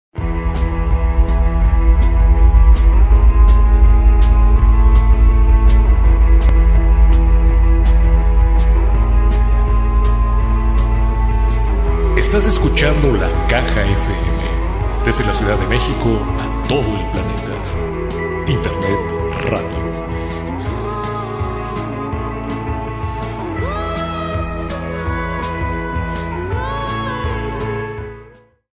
La Caja FM / Internet Radio (HD)
LaCaja.FM es un proyecto de Internet Radio / Televisión que se transmite desde la Ciudad de México al mundo entero por medio de las ondas del cyber espacio.
Una estación que te acerca a lo mejor de la música independiente y sus creadores.